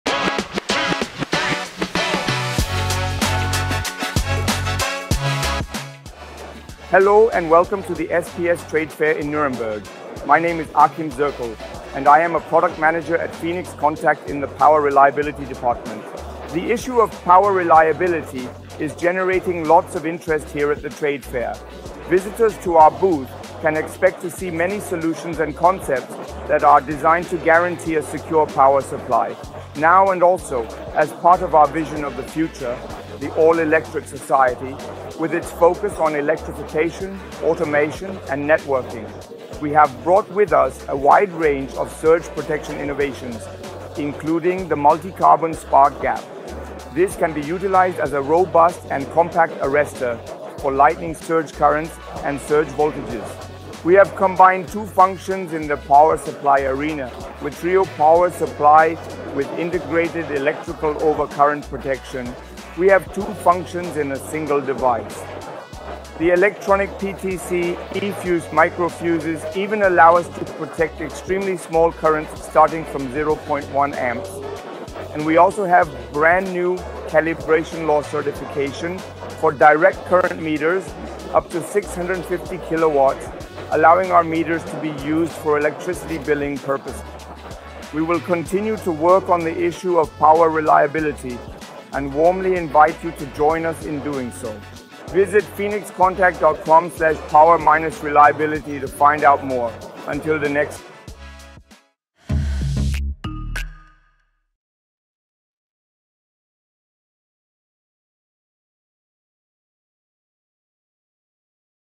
SPS 2023 - Power Reliability
►Electrification, networking, automation: That was the SPS 2023!

►Achim Zirkel reports from the SPS on our solutions to ensure a secure power supply in the All Electric Society.

►Our products and solutions are paving the way toward a climate-neutral and sustainable future.